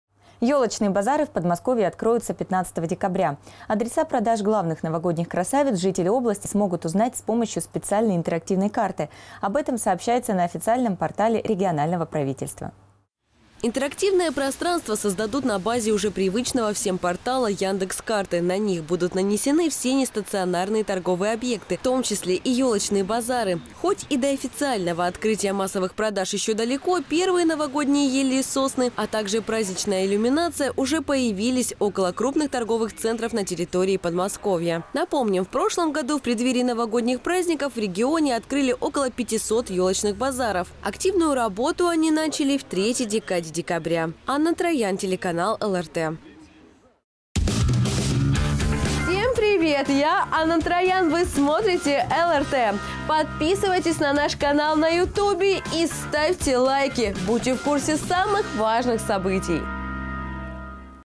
Елочные базары начнут работу с 15 декабря
Елочные базары в Подмосковье откроются 15 декабря.  Адреса продажи главных новогодних красавиц жители области смогут узнать с помощью специальной интерактивной карты. Об этом сообщается на официальном портале регионального правительства.

Интерактивное пространство создадут на базе уже привычного всем портала «Яндекс.Карты». На них будут нанесены все нестационарные торговые объекты, в том числе, и елочные базары.

Хоть и до официального открытия массовых продаж еще далеко, первые новогодние ели и сосны, а также праздничная иллюминация уже появились около крупных торговых центров на территории Подмосковья.

Напомним, в прошлом году в преддверии новогодних праздников в регионе открыли около пятисот елочных базаров. Активную работу они начали в третьей декаде декабря.

Анна Троян